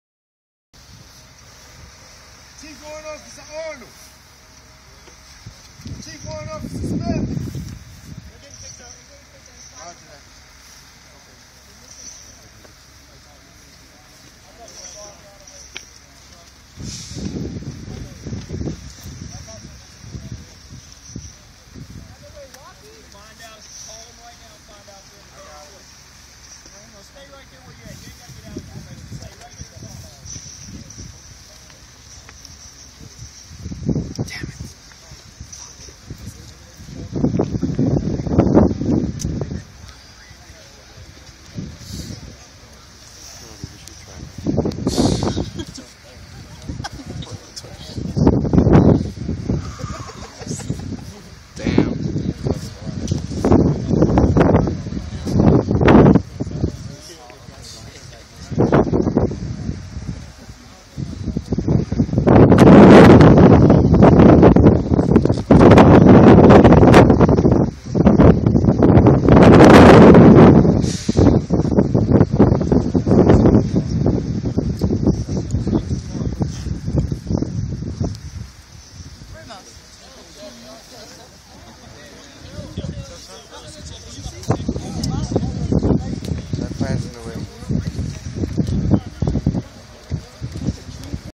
Land Nav Weather
This is the crap they had us doing Land Nav training in. It sucked. -.-